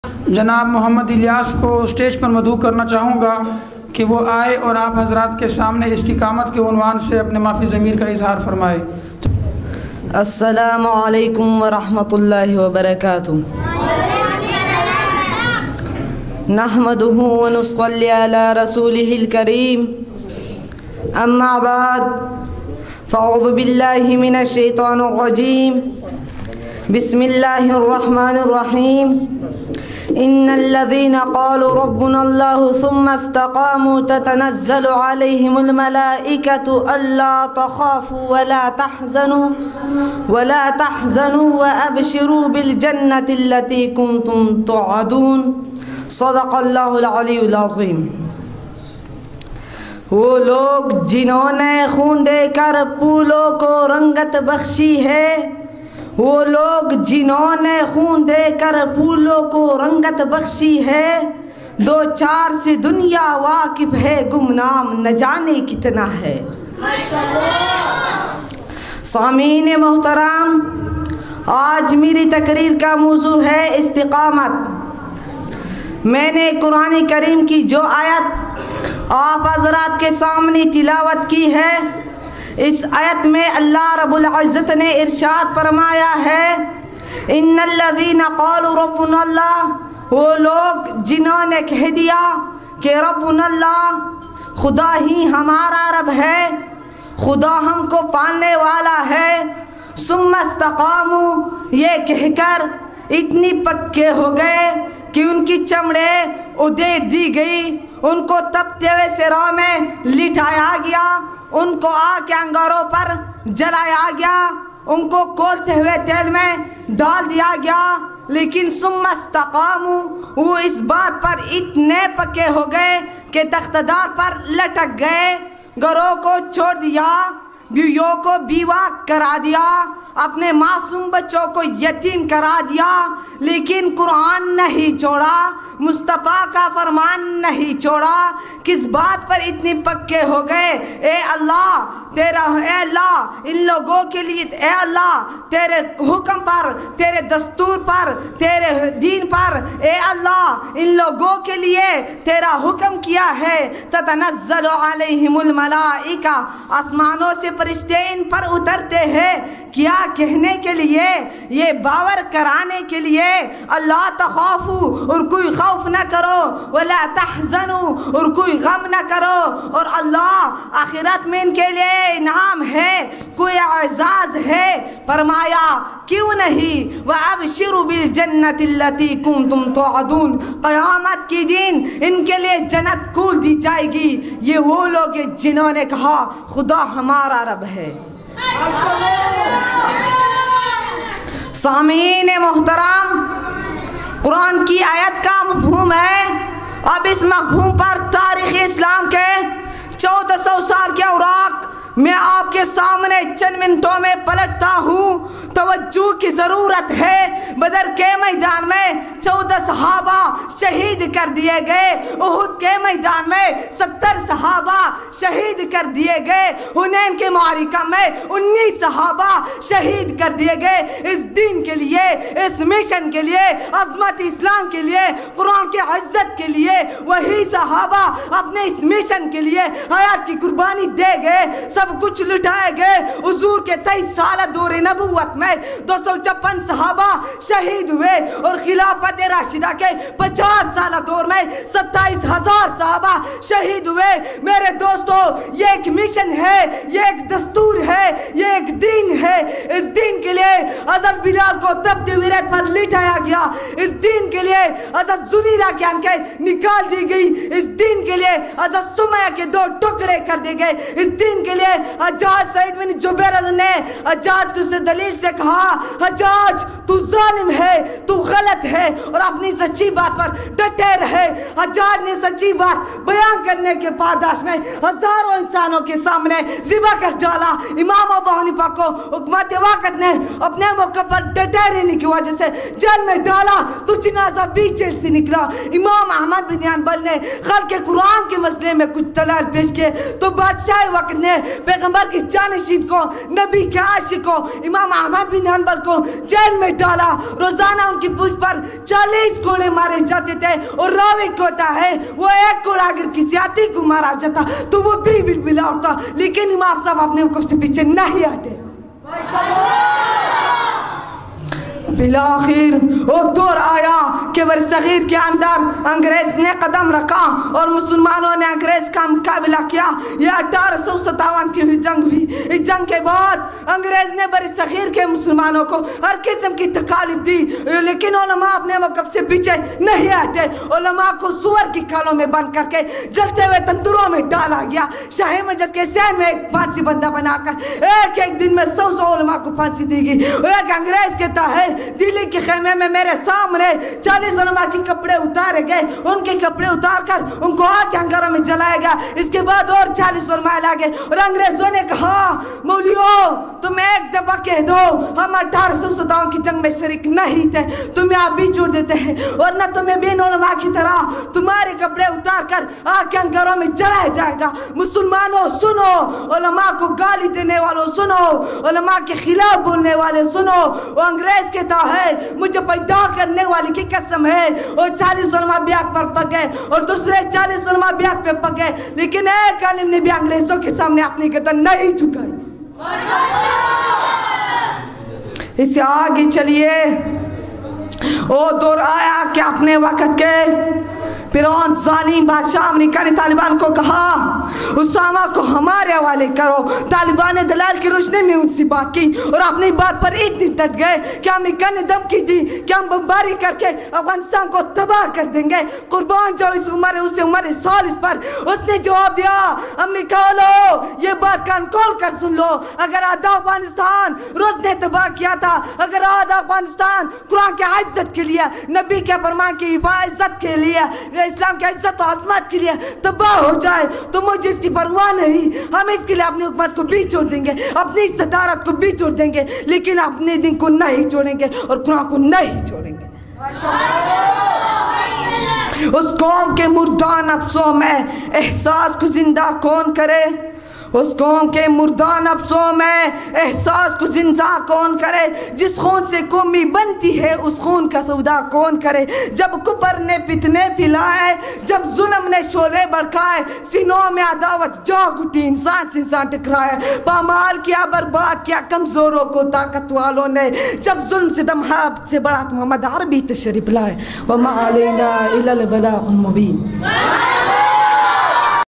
سالانہ دستارفضیلت وتقسیم انعامات/مدرسہ کے طالب علم محمد الیاس/موضوع استقامت/15جنوری2023
بمقام جامعہ انوار المصطفٰی گلی نمبر 4 سیکٹر7C نارتھ کراچی